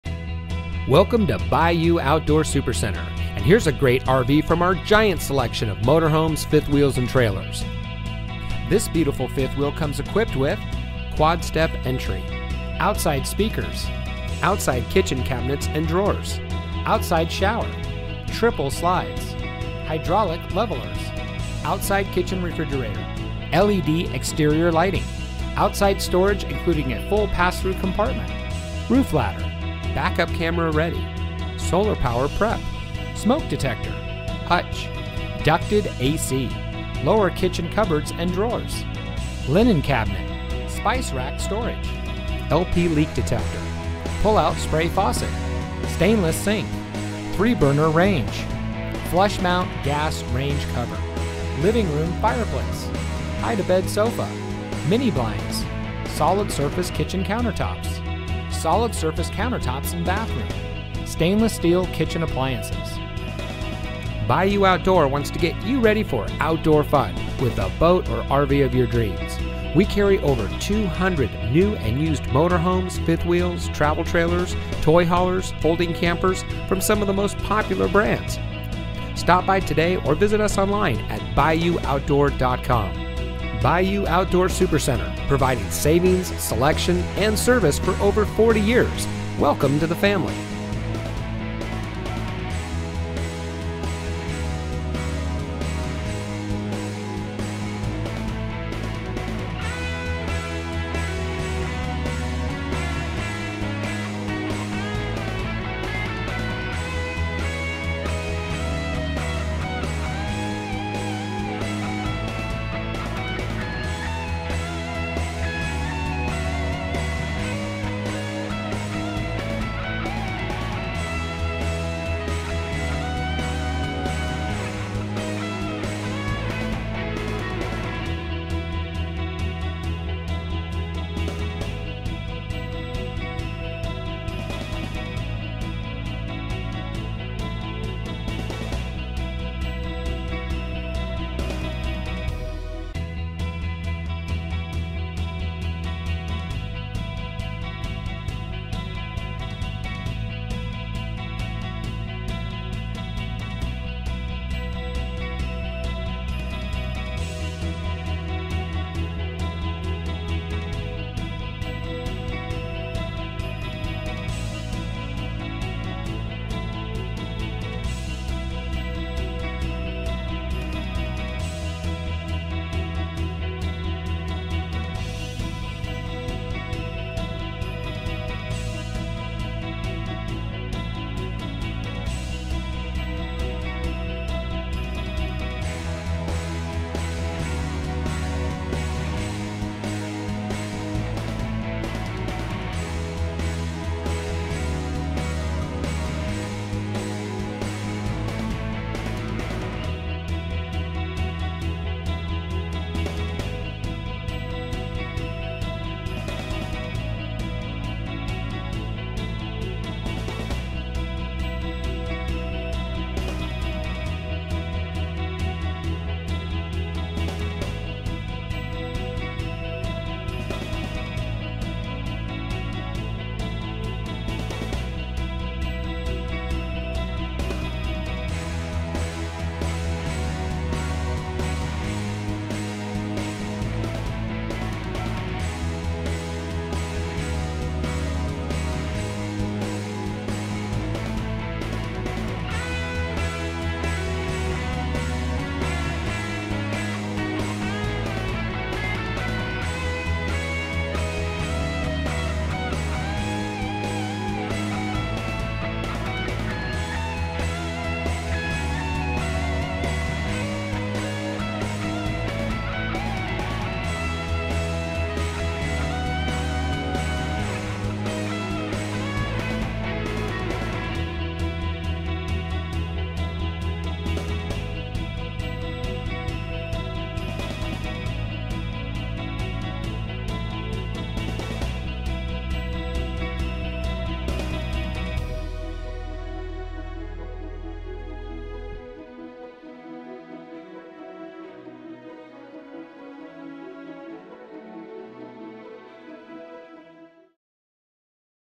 2021 Forest River CARDINAL LIMITED 366DVLE Fifth Wheel For Sale In Bossier Near Shreveport,Louisiana
2021 Forest River CARDINAL LIMITED 366DVLE Stock Number: 22396 Vin:22396

Bayou Outdoor
1800 Barksdale Blvd.